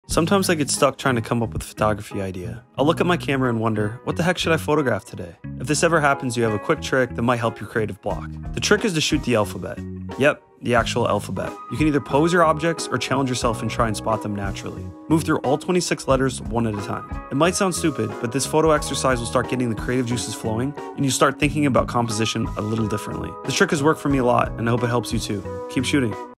🤔 No Photo ideas? Try this! 📸
Get your creative juices flowing with this alphabet photography challenge! Take a photo of objects around your house in the shape of each letter of the alphabet. From "A" to "Z", this fun and playful activity is a great way to explore your environment and unleash your creativity. Watch now for inspiration.